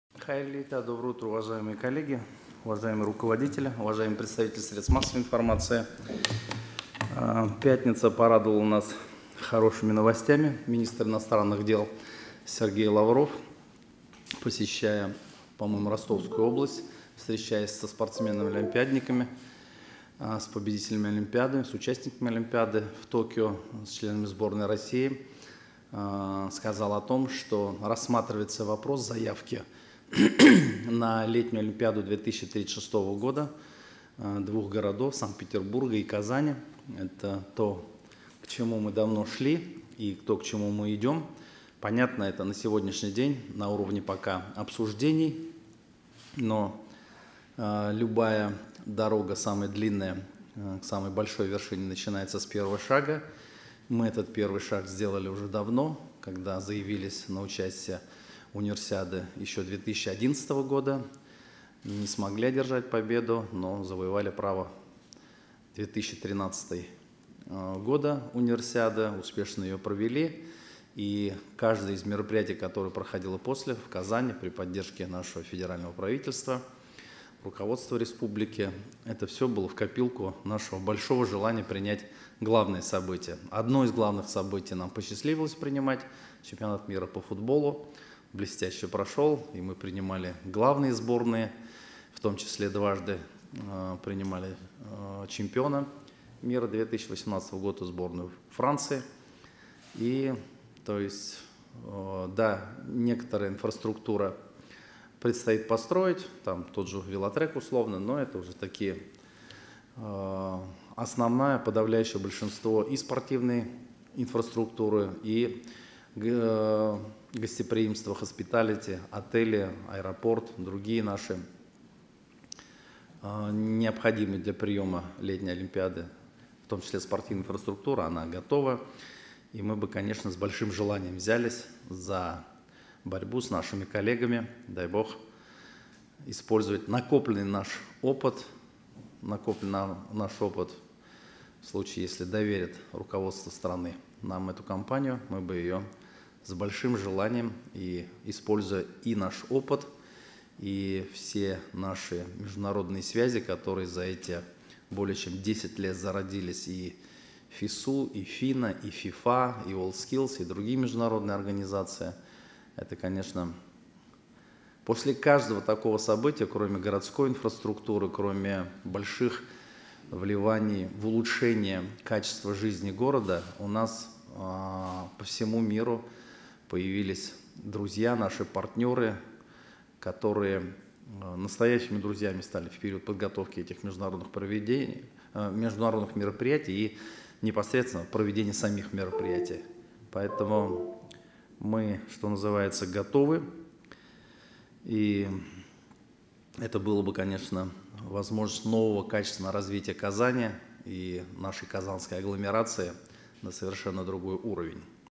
Мэр Казани заявил о готовности бороться за право проведения Олимпиады-2036
Источник: Официальный портал мэрии Казани kzn.ru